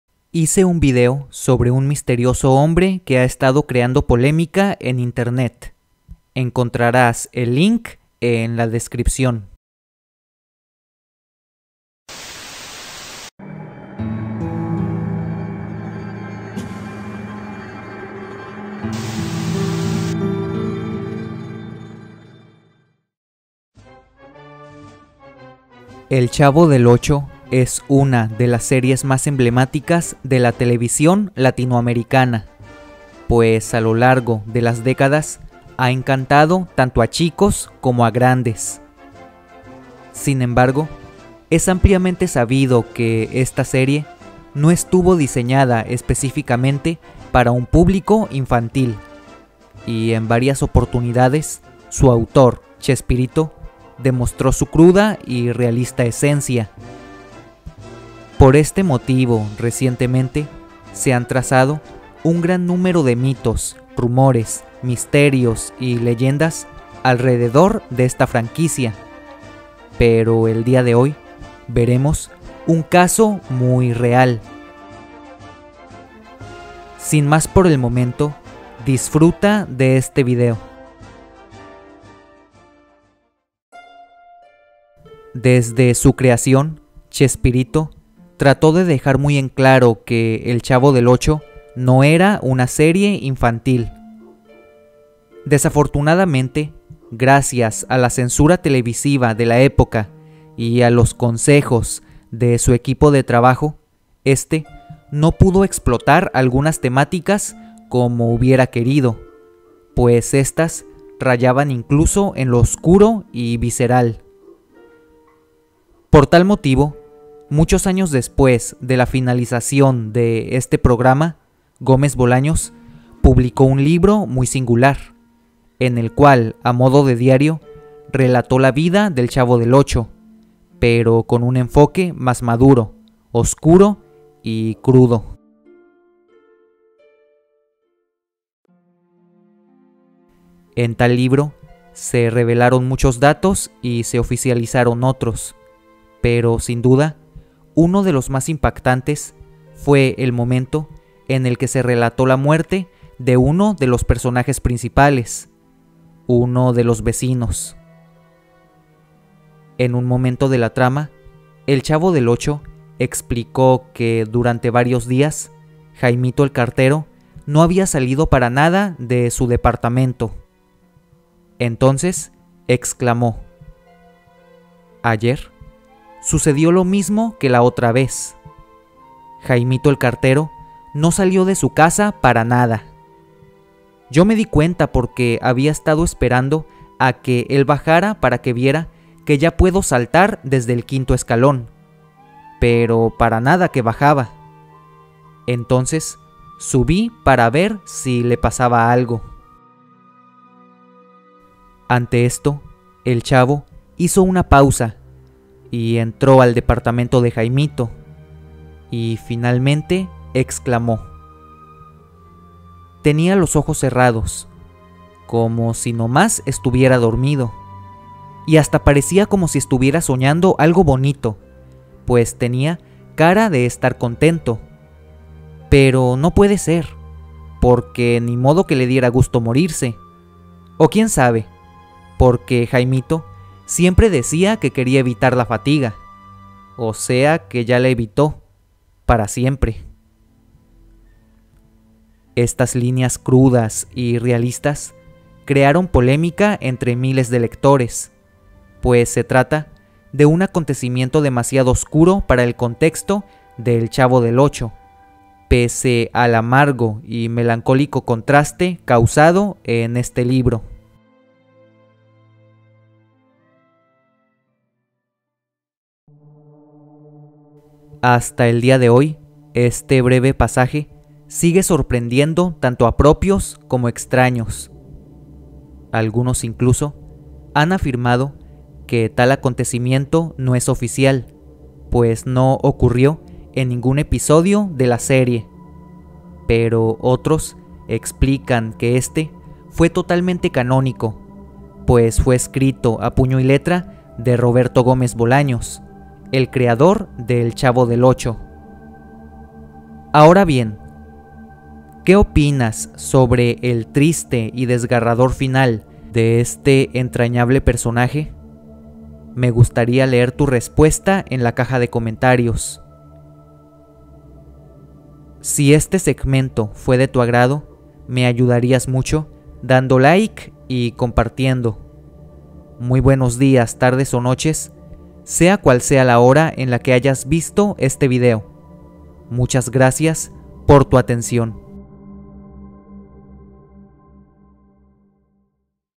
NO Creerás el Trágico FINAL de este Personaje del Chavo del 8
El Chavo del 8 es una de las series más emblemáticas de Latinoamérica. Sin embargo, por este mismo motivo, esta está rodeada de teorías y misterios bastante perturbadores. El día de hoy, analizaremos un caso al respecto, que por cierto, es completamente real.